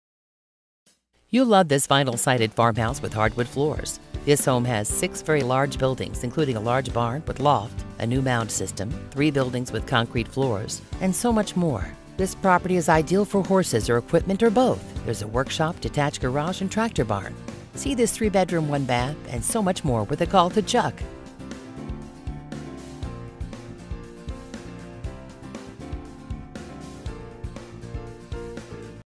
8608 Tri County Hwy Eagle Twp, OH | MLS# 1548466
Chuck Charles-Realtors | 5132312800

Vinyl sided farmhouse with hardwood floors. This home has 6 very large buildings, including large barn w/loft. New mound system. 3 buildings w/concrete floors. Ideal for horses or equipment or both! Workshop, detached garage and tractor barn.